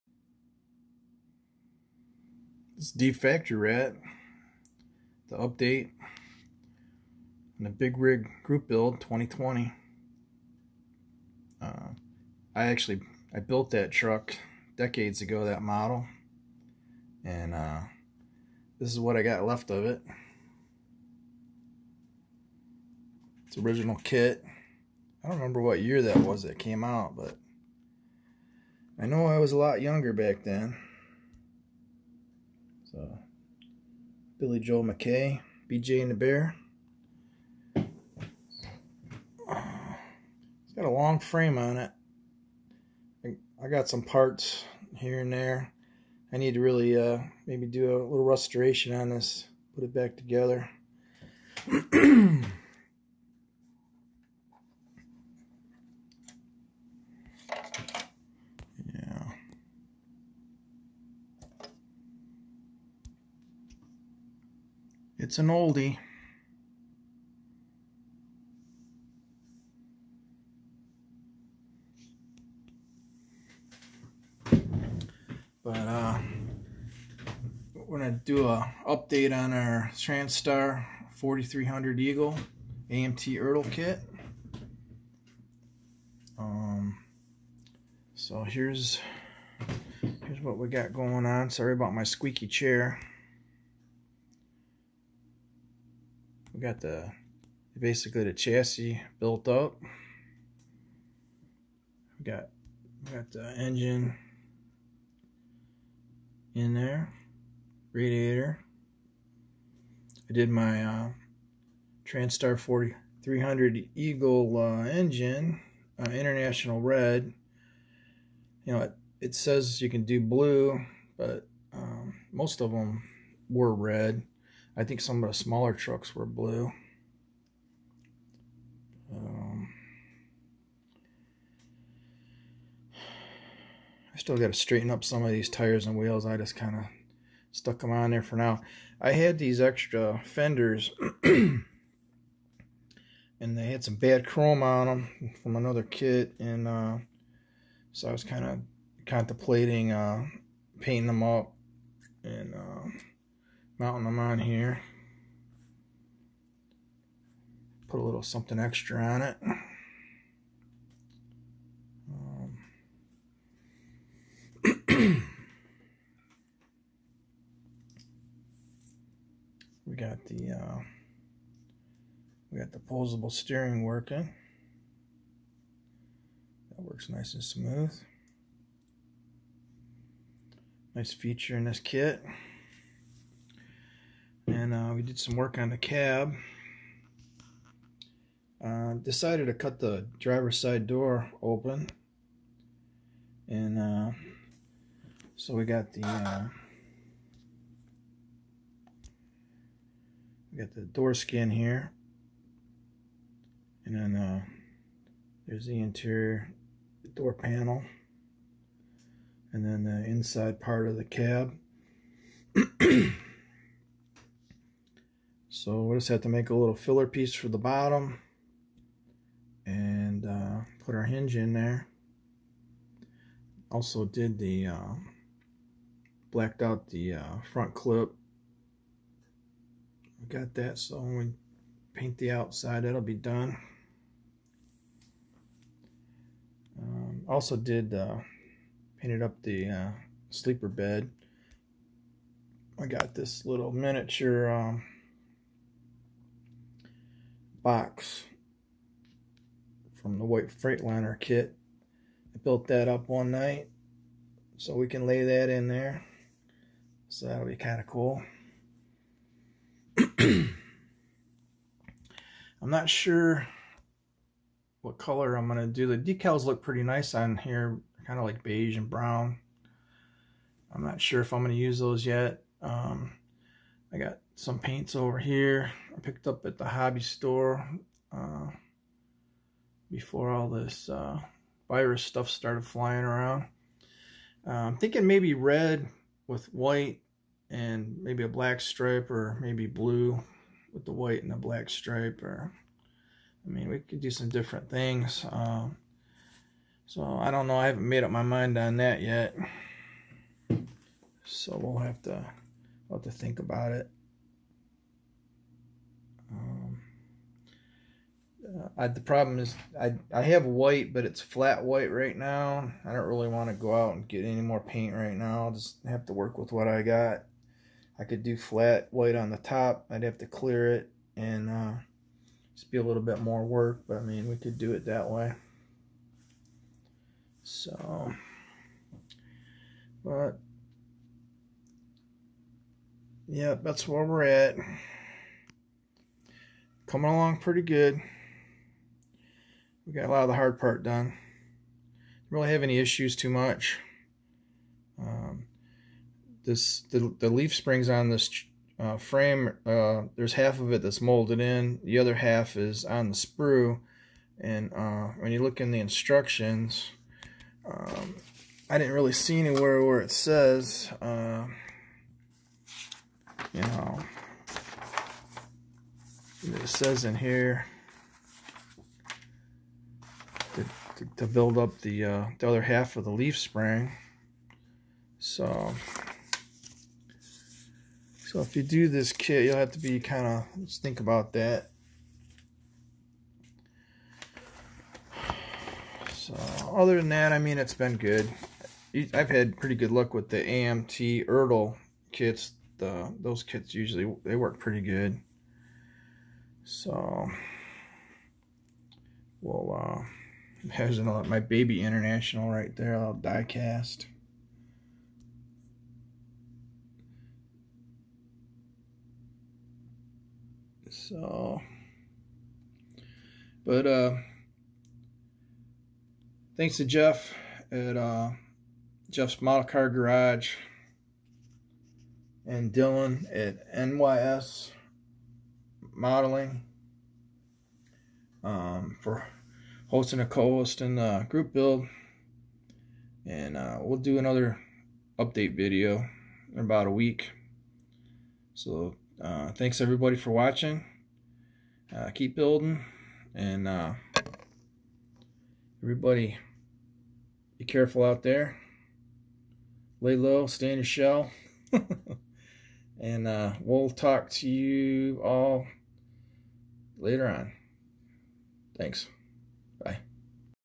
Big Rig Group Build 2020. ( I.H.) Transtar 4300 Eagle 🦅 Update
Mature content (requested by uploader) Things are building up pretty well. I decided to cut open the driver's door. The sleeper on the drivers side also opens. So I think that will look nice to see the doors displayed open. Next I will have to comment to paint scheme & get that going. Also building a hinge for the door. Thanks for checking out my progress 🐀